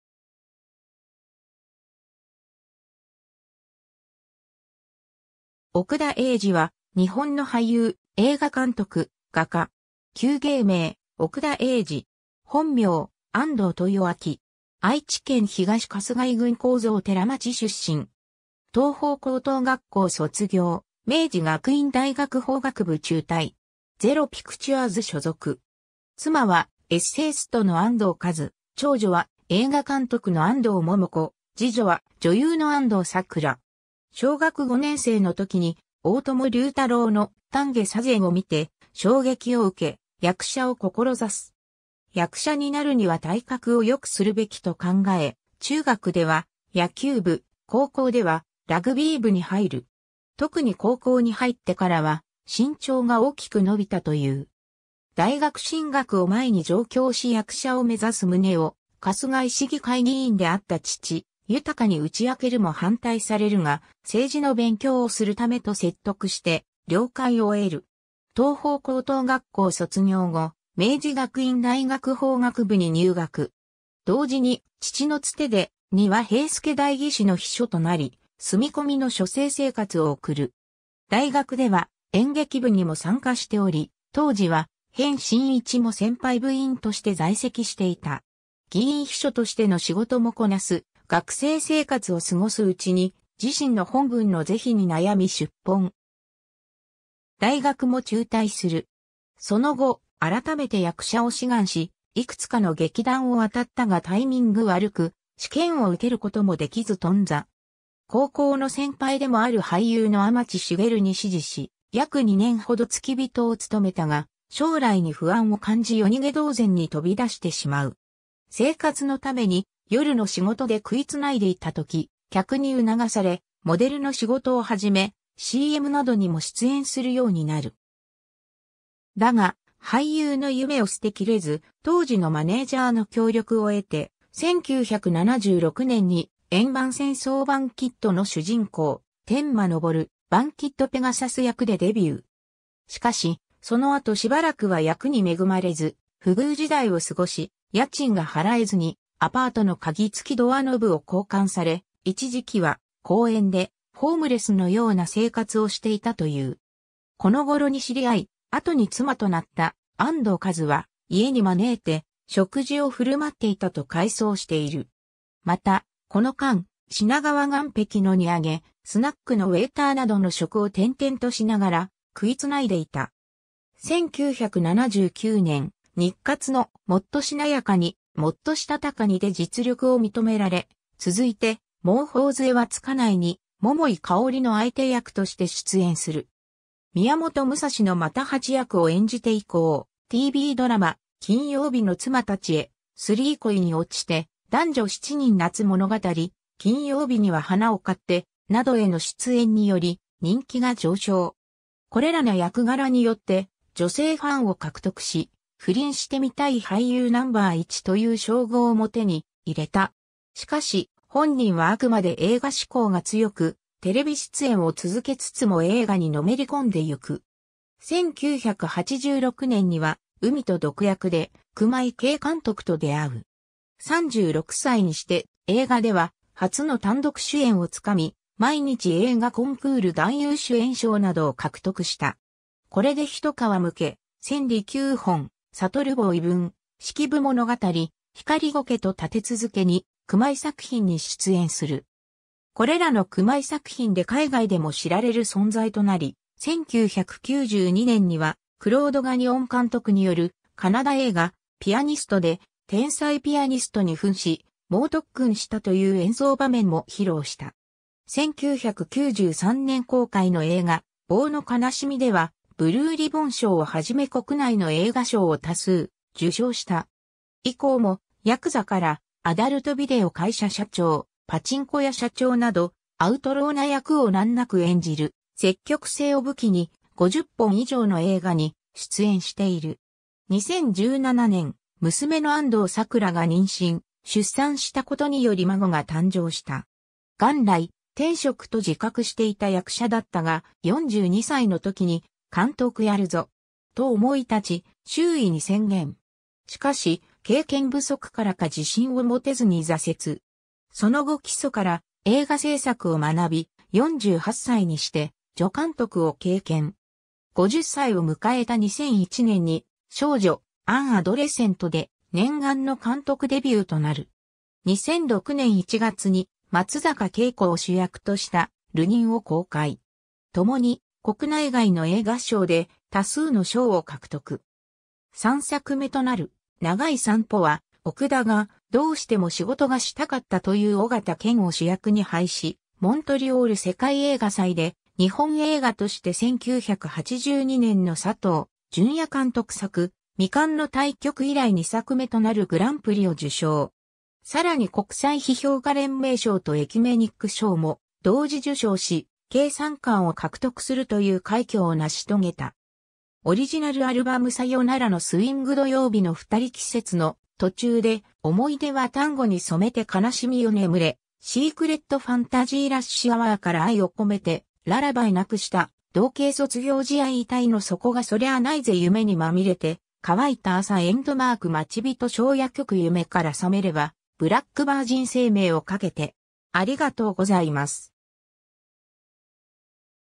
奥田瑛二
奥田・安藤家
奥田 瑛二（おくだ えいじ、1950年3月18日 - ）は、日本の俳優・映画監督・画家。
旧芸名：奥田 英二（読み同一）。
本名：安藤 豊明（あんどう とよあき）。
 愛知県東春日井郡高蔵寺町（現：春日井市）出身。
東邦高等学校卒業、明治学院大学法学部中退。
ゼロ・ピクチュアズ所属。
妻はエッセイストの安藤和津、長女は映画監督の安藤桃子（旧名・安藤モモ子）、二女は女優の安藤サクラ。
 小学5年生のときに大友柳太朗の『丹下左膳』を観て衝撃を受け、役者を志す。
役者になるには体格を良くするべきと考え、中学では野球部、高校ではラグビー部に入る。
特に高校に入ってからは身長が大きく伸びたという。
 大学進学を前に上京し役者を目指す旨を春日井市議会議員であった父・豊に打ち明けるも反対されるが、「政治の勉強をするため」と説得して了解を得る。
東邦高等学校卒業後、明治学院大学法学部に入学。
同時に父の伝で丹羽兵助代議士の秘書となり、住みこみの書生生活を送る。
大学では演劇部にも参加しており、当時は辺真一も先輩部員として在籍していた。
議員秘書としての仕事もこなす学生生活を過ごすうちに自身の本分の是非に悩み出奔。
大学も中退する。
その後、改めて役者を志願し、いくつかの劇団を当たったがタイミング悪く、試験を受けることも出来ず頓挫。
高校の先輩でもある俳優の天知茂に師事し、約2年ほど付き人を務めたが、将来に不安を感じ夜逃げ同然に飛び出してしまう。
 生活のために夜の仕事で食いつないでいたとき、客に促されモデルの仕事を始め、CMなどにも出演するようになる。
だが、俳優の夢を捨てきれず、当時のマネージャーの協力を得て、1976年に『円盤戦争バンキッド』の主人公・天馬昇 / バンキッドペガサス役でデビュー。
しかし、その後しばらくは役に恵まれず不遇時代を過ごし、家賃が払えずにアパートの鍵付きドアノブを交換され、一時期は公園でホームレスのような生活をしていたという。
この頃に知り合い、後に妻となった安藤和津は、家に招いて食事を振る舞っていたと回想している。
また、この間、品川岸壁の荷揚げ、スナックのウェイターなどの職を転々としながら、食い繋いでいた。
 1979年、日活の『もっとしなやかに、もっとしたたかに』で実力を認められ、続いて『もう頬づえはつかない』に桃井かおりの相手役として出演する。
『宮本武蔵』の又八役を演じて以降、TVドラマ『金曜日の妻たちへIII 恋におちて』（1985年）、『男女7人夏物語』（1986年）、『金曜日には花を買って』（1986年 - 1987年）などへの出演により人気が上昇。
これらの役柄によって女性ファンを獲得し、「不倫してみたい俳優」ナンバー1という称号も手に入れた。
しかし、本人はあくまで映画志向が強く、テレビ出演を続けつつも映画にのめり込んでゆく。
 1986年には『海と毒薬』（ベルリン映画祭銀熊賞）で熊井啓監督と出会う。
36歳にして映画では初の単独主演をつかみ、毎日映画コンクール男優主演賞などを獲得した。
これでひと皮むけ、『千利休 本覺坊遺文』（1989年、ベネチア映画祭銀獅子賞）、『式部物語』（1990年、モントリオール映画祭芸術貢献賞）、『ひかりごけ』（1992年）と立て続けに熊井作品に出演する。
これらの熊井作品で海外でも知られる存在となり、1992年にはクロード・ガニオン監督によるカナダ映画『ピアニスト』で天才ピアニストにふんし、猛特訓したという演奏場面も披露した。
 1993年公開の映画『棒の哀しみ』ではブルーリボン賞を始め国内の映画賞を多数受賞した。
以降も、ヤクザからアダルトビデオ会社社長、パチンコ屋社長など、アウトローな役を難無く演じる積極性を武器に、50本以上の映画に出演している。
 2017年、娘の安藤サクラが妊娠・出産したことにより孫が誕生した。
 元来「天職」と自覚していた役者だったが、42歳のときに「監督やるぞ!」と思い立ち、周囲に宣言。
しかし、経験不足からか自信を持てずに挫折。
その後基礎から映画製作を学び、48歳にして助監督を経験。
50歳を迎えた2001年に『少女〜AN ADOLESCENT』で念願の監督デビューとなる。
 2006年1月に松坂慶子を主役とした『るにん』を公開。
共に国内外の映画賞で多数の賞を獲得。
3作目となる『長い散歩』は...